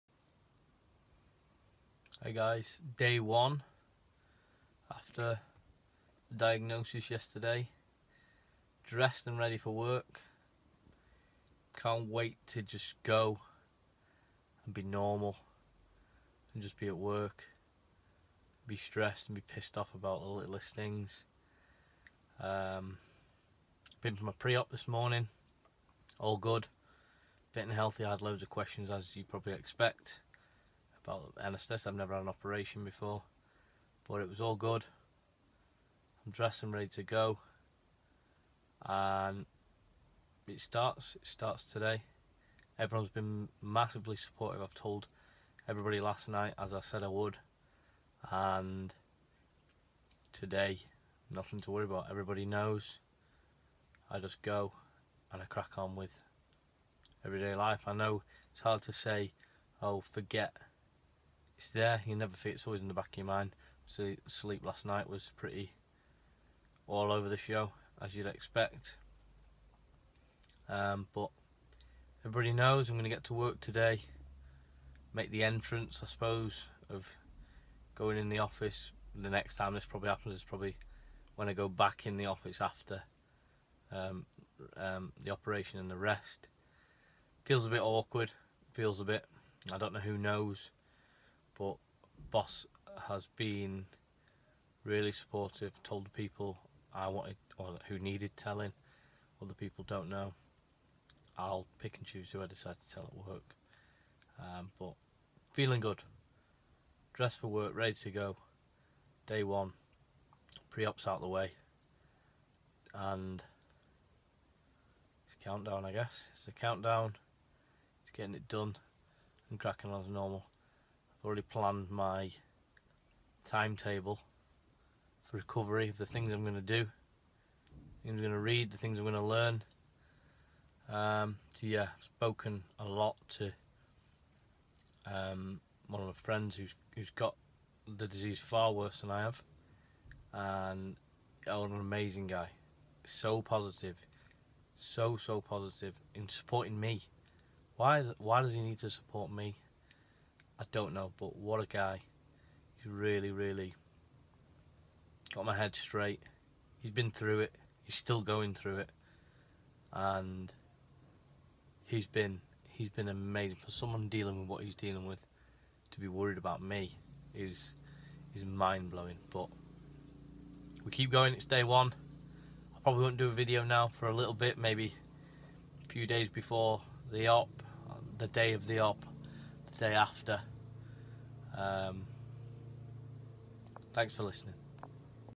Cancer blog - My story: Day 1 following diagnosis
This is day 1 video following the diagnosis. I hope these videos help you in any way